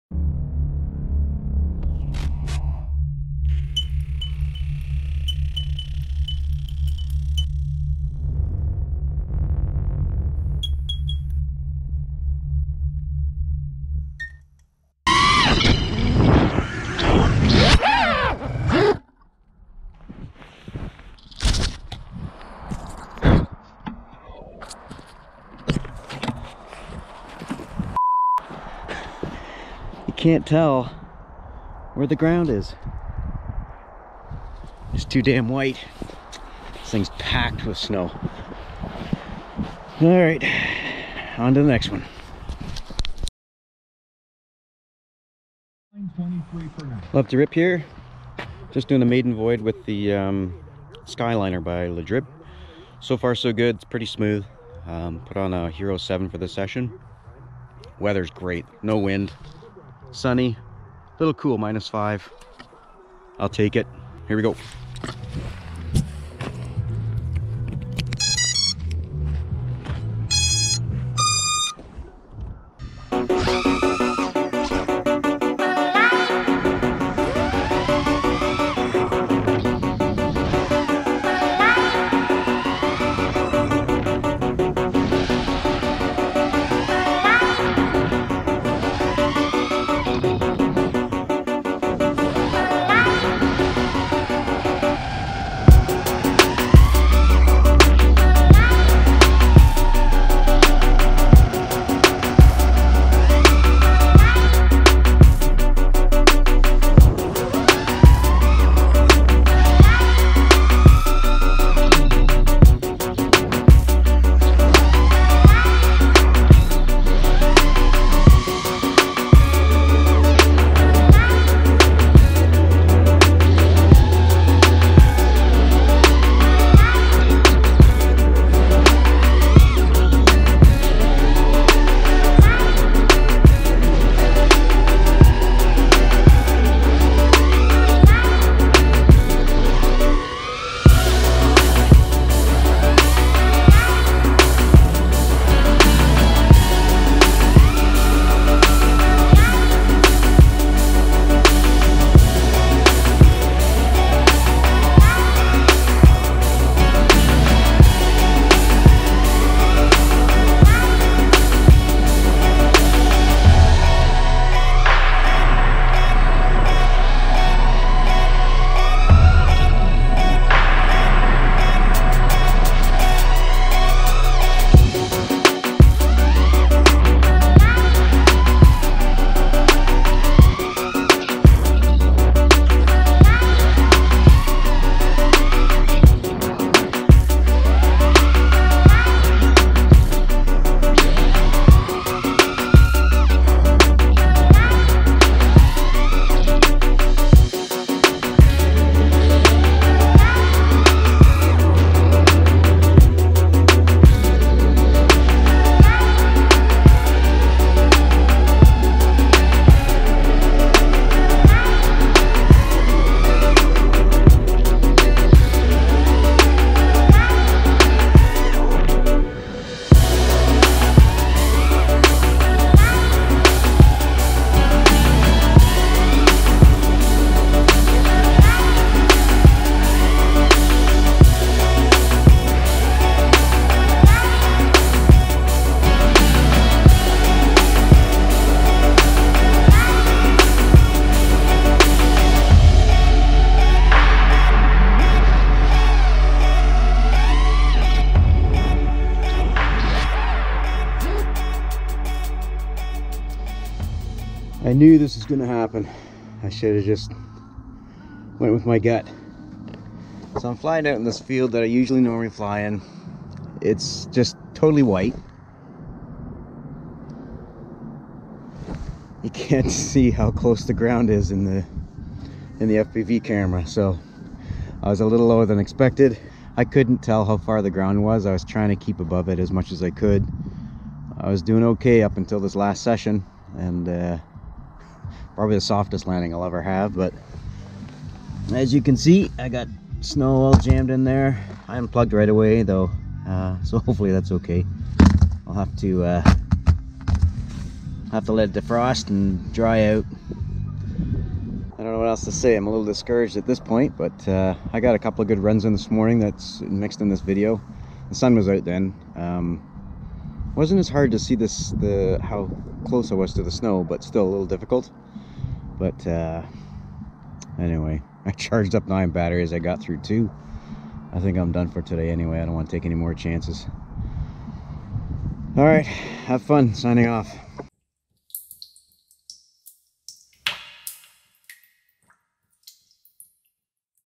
I Did A Face Plant In The Snow!
Damn it was just too white with all the snow!!!  The ground got washed out and I misjudged the distance. It's probably the softest landing I will ever have though!

Equipment:

QUAD
Xhover Skyeliner LeDrib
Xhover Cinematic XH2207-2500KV
DYS F4 FC
Immersion 5.8 VTX with Smart Audio 25/600mw
XII U.FL 5.8 FPV Antenna
RX - FrSky
HQ Ethix Props
Rotor Riot RunCam camera

TRANSMITTER
FrSky Horus X10S

FPV GOGGLES
Fatshark HDO
Immersion RC Rapidfire